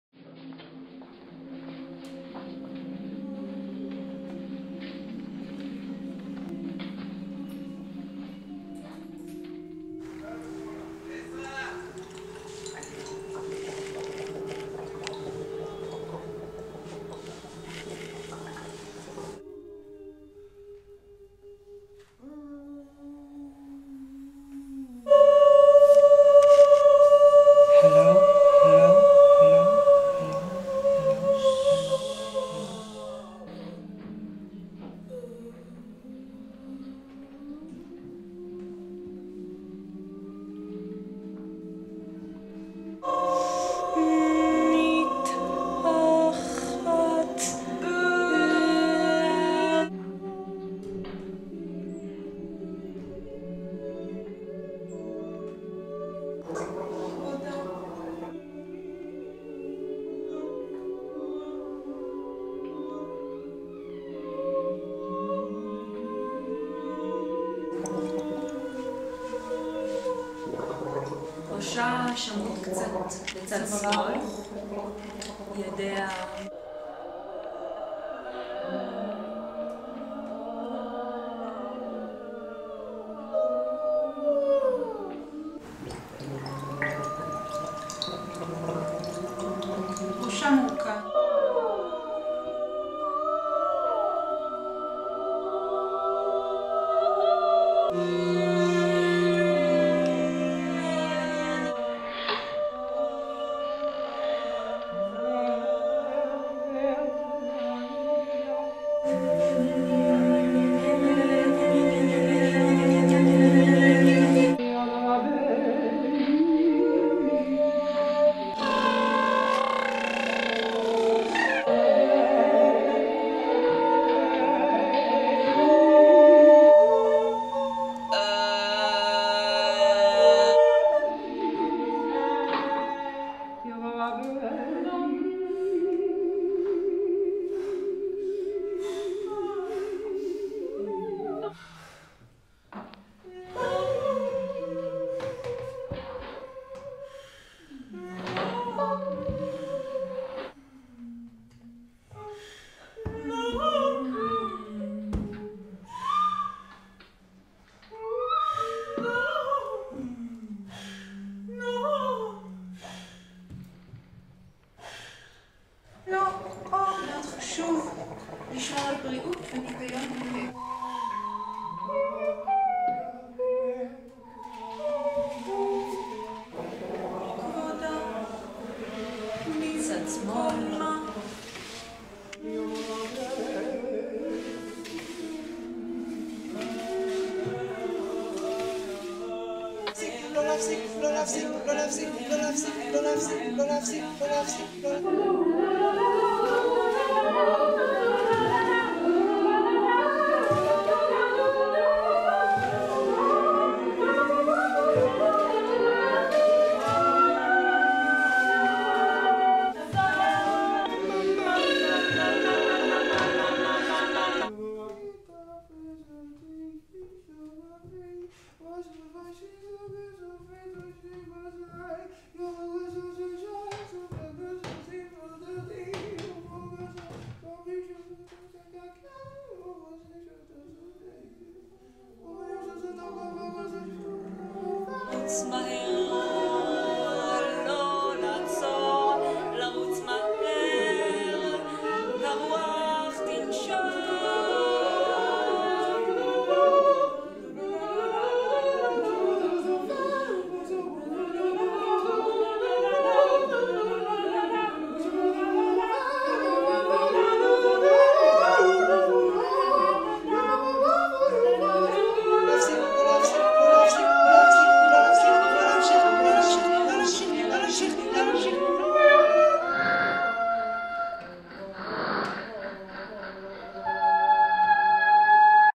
abraBARBUR ברבוראברא
מתוך הופעה אחרונה בגלרית ברבור וסשן אלתור בבית. "אברא כדברא" היא הרכב תיאטרון אלתור קולי. משתתפים: נעה הרן, פיי שפירו, יפעת זיו וחנן בן סימון.
עריכה: פיי שפירו. צילום: אור סיני ומיה ברן. סאונד: לארס סרגל.
תודה לאילת לרמן ולהאזנות סתר.

Excerpts from a show at Barbur Gallery in Jerusalem, and from a home session.
ABRAkadabra is a voice-theatre imrov ensemble/ by Noa Haran, Chanan Ben Simon, Yifeat Ziv and Faye Shapiro. Sound By Lars Sergel, Video shot by Maya Baran and Orr Sinai, Video Editing by Faye Shapiro.
 .We thank Ayelet Lerman for the Wire Tapping